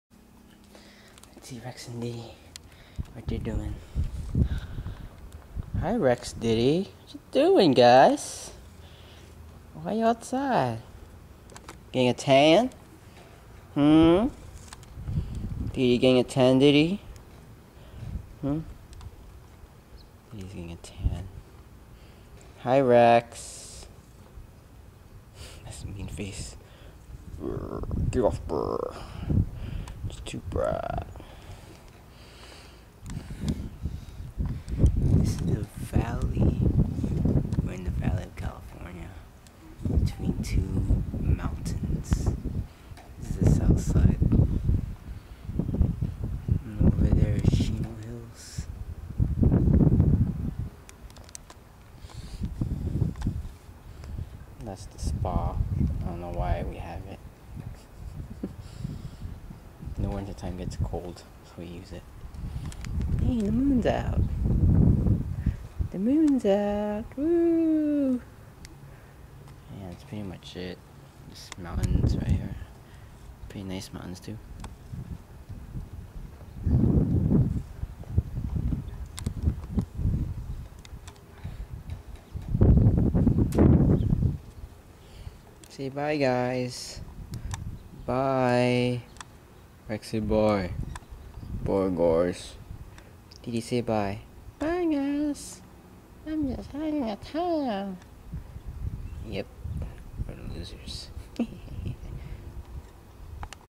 rex and diddy in the backyard
rex and diddy having some fun under the sun